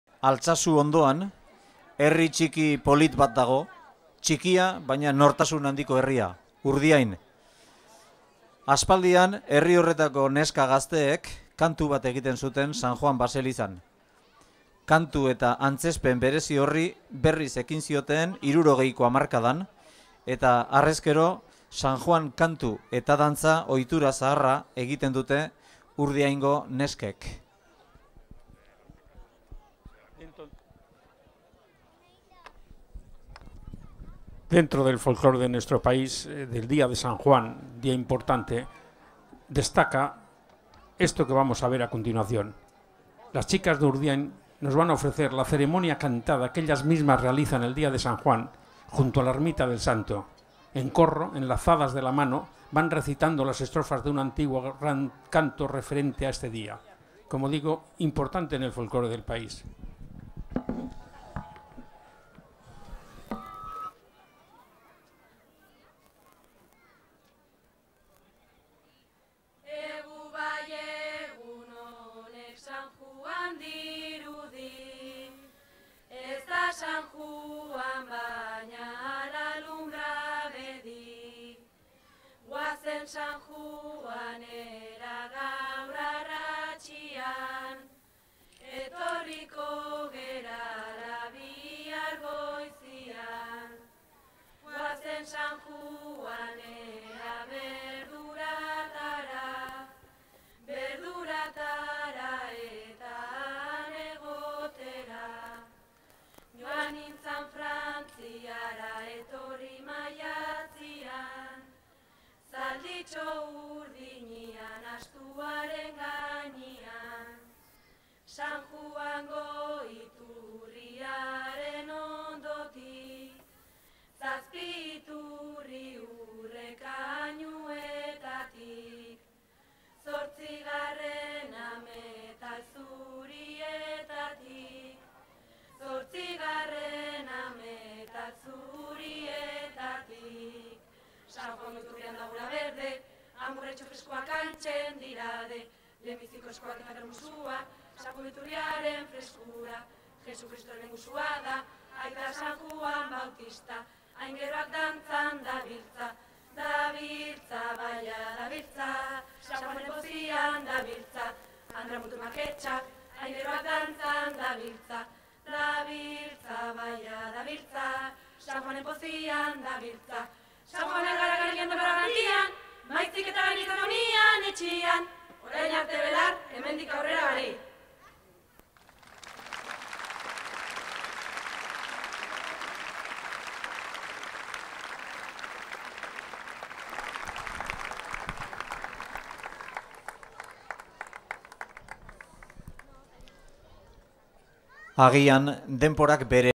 SAN JUAN KANTU ETA DANTZA - Urdiain 2008
Euskal Herriko Dantza Agerketa - 2008
URDIAINgo neskak